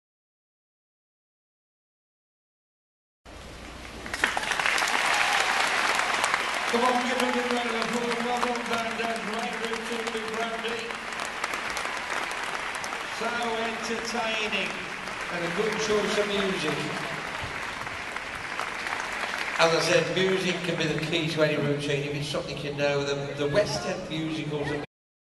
Online reportáž DOGS magazin z Crufts 2012
DOGSmagazín vám přináší ONLINE reportáž z psí výstavy CRUFTS 2012 z anglického Birminghamu.